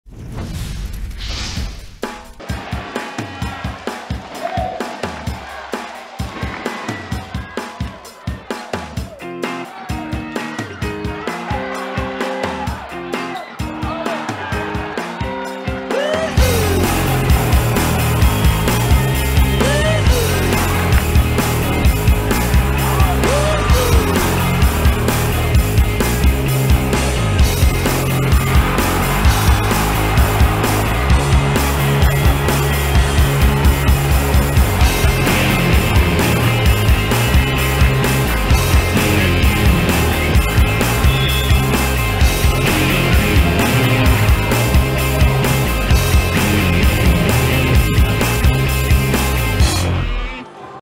Вот так выигрывают матчи! / Fakel - Berlin Recycling Volleys best moment
Самый захватывающий розыгрыш матча "ФАКЕЛ" - "Берлин", который решил исход первого сета.